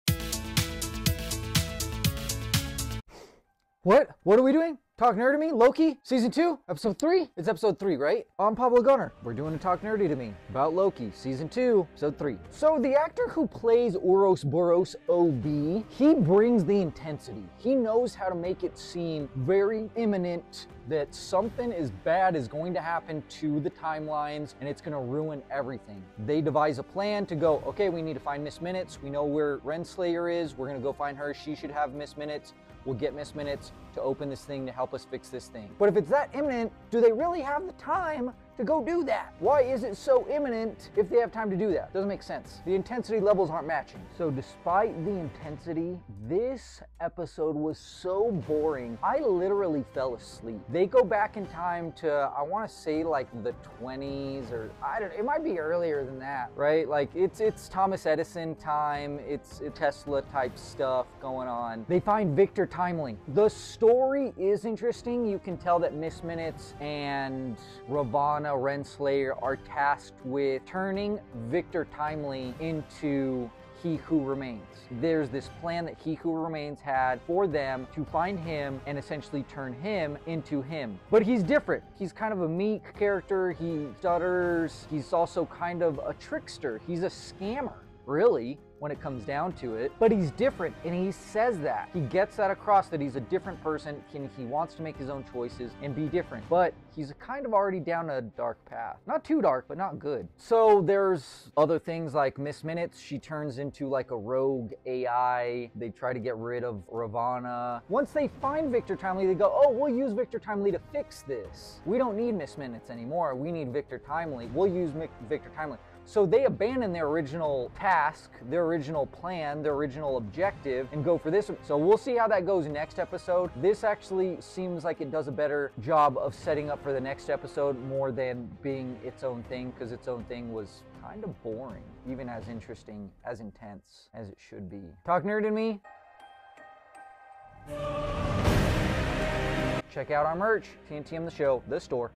Talk Nerdy To Me LOKI Season 2 EPISODE 3 by Pablo Gunner
Pablo Gunner, delivers a Talk Nerdy To Me review for the LOKI Season 2 EPISODE 3, as a fan of Marvel comics, movies, shows, and games.